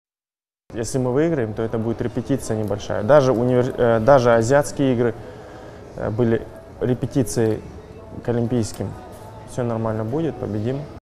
Серик Сапиев: все будет нормально, победим!
В столице Малайзии через несколько часов начнется процедура голосования, которая определит хозяйку главных игр четырехлетия Зимней Олимпиады -2022.

Все спортсмены перед важными соревнованиями верят в суеверия, а также соблюдают все свои спортивные ритуалы. Так и перед оглашением столицы белой Олимпиады. Первое правило не быть голодным. Поэтому наши спортсмены, которые сейчас находятся в центре событий в Куала-Лумпур, по традиции пошли в кафе для того, чтобы основательно подкрепиться.

Волнительного события с нетерпением ждет Олимпийский чемпион по боксу Серик Сапиев. У спортсменов есть суеверия: не давать интервью накануне важных стартов. Но для нас было сделано исключение.

Серик Сапиев, Олимпийский чемпион по боксу

- Если мы вы играем это можно сказать будет репетиция. Даже азиатские игры были репетицией. Все нормально будет, победим!